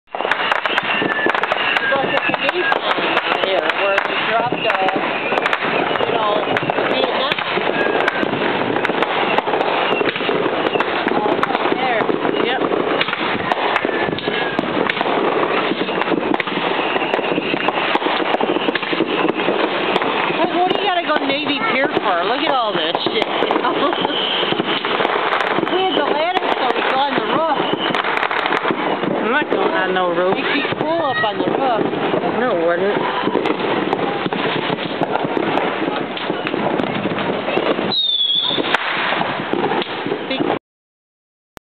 Fireworks Madness Chicago
Just a couple of seconds of fireworks on the Fourth of July, 2010